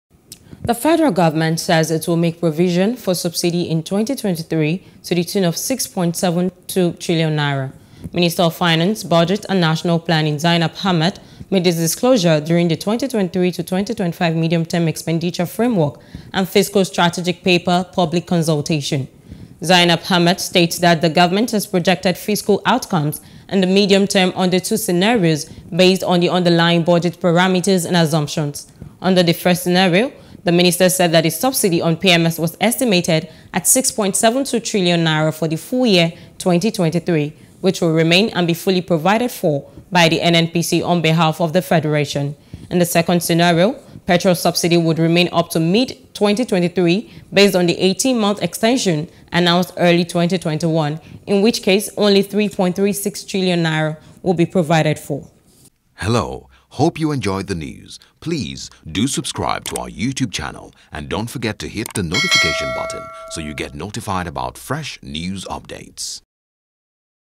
Subsidy: FG To Spend N6.72TRN In 2023 | NEWS
The Federal Government says it will make provision for subsidy in 2023 to the tune of N6.72 trillion.